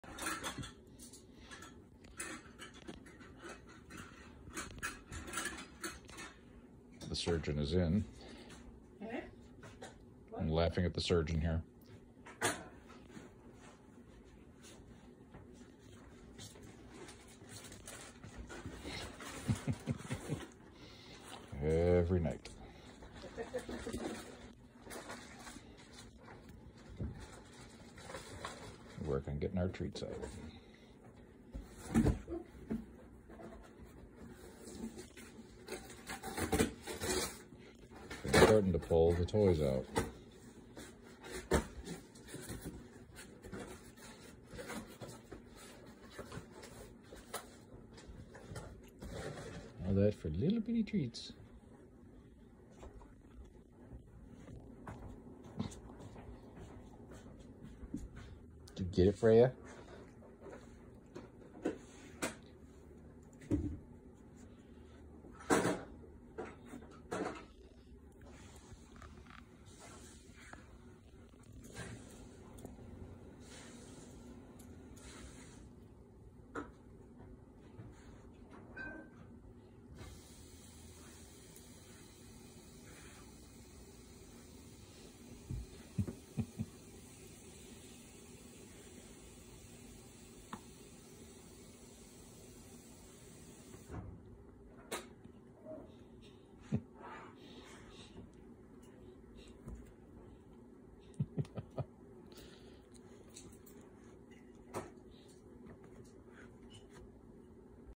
Freja the surgeon part 1
Nothing stops Freja from getting her treats